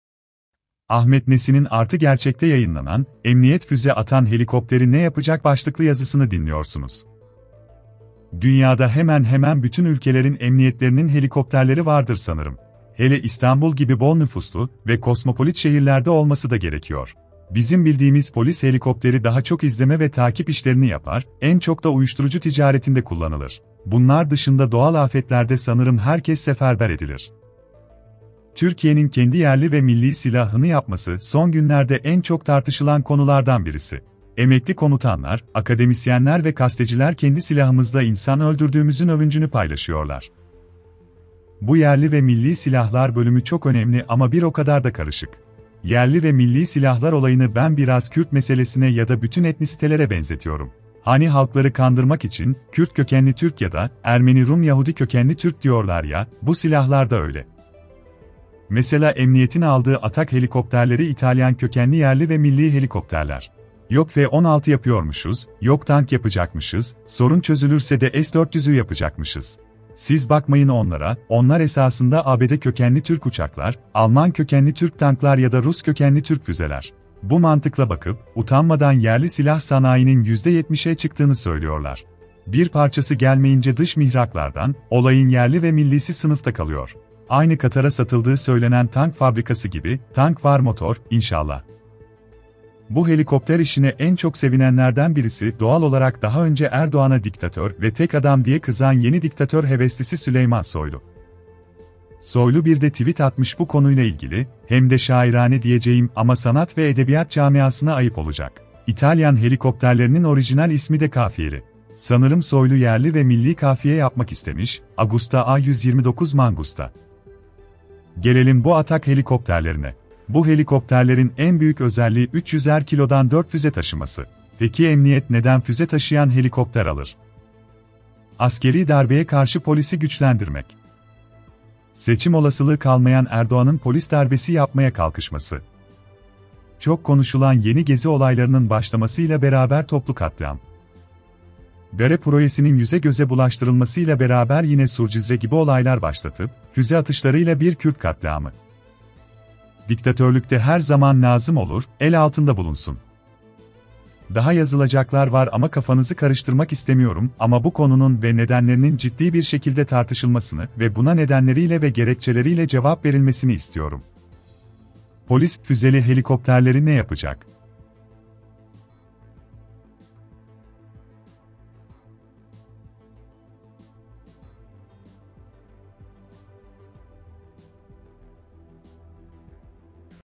Ahmet Nesin - Emniyet füze atan helikopteri ne yapacak!..
Dünyada hemen hemen bütün ülkelerin emniyetlerinin helikopterleri vardır sanırım. Hele İstanbul gibi bol nüfuslu ve kosmopolit şehirlerde olması da gerekiyor. Bizim bildiğimiz polis helikopteri daha çok izleme ve takip işlerini yapar, en çok da uyuşturucu ticaretinde kullanılır. Bunlar dışında doğal afetlerde sanırım herkes seferber edilir.